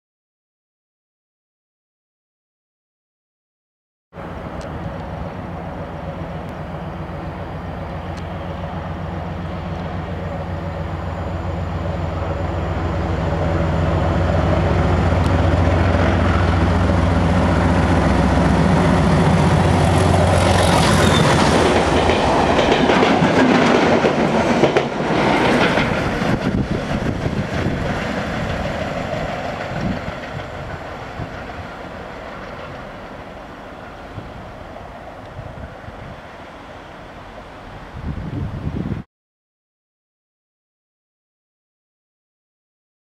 3Z02 Network Rail 31465 & 9702 Bristol High Level Siding to Derby R.T.C(Network Rail)
31465 & 9702 on 3Z02 1011 Bristol High Level Siding to Derby R.T.C.(Network Rail), Test Train seen passing Water Orton about 6 mins early.